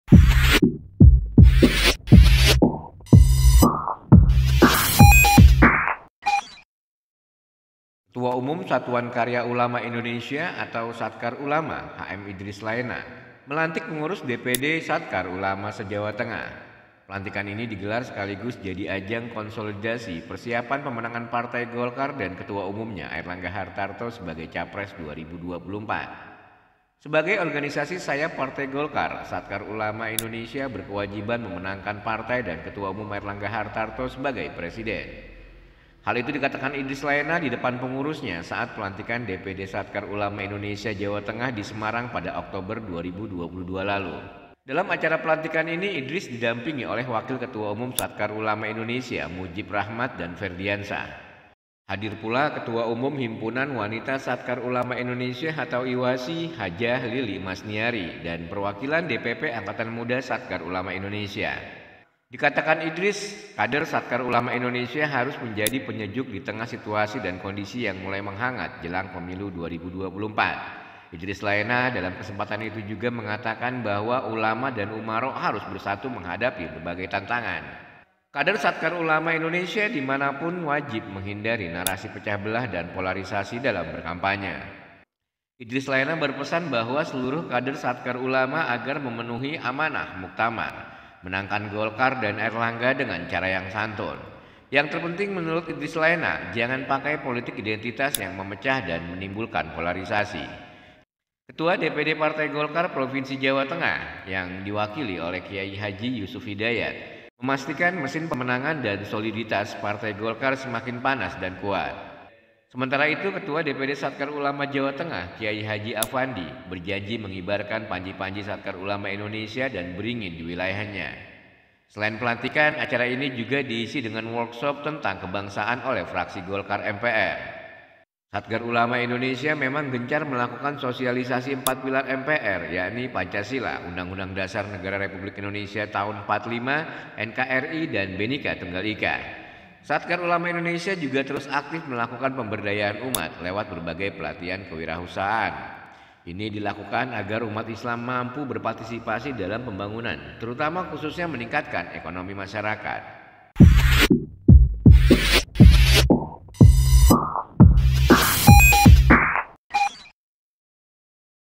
Idris Laena: Satkar Ulama Wajib Menangkan Golkar dan Airlangga
Ketua Umum Satkar Ulama Indonesia, Idris Laena menginstruksikan kepada seluruh kader dan simpatisan untuk mendukung pencapresan Airlangga Hartarto di Pilpres 2024. Bagi Idris Laena, memenangkan Airlangga Hartarto dan Partai Golkar di Pemilu 2024 menjadi tugas mutlak yang harus dijalankan Satkar Ulama.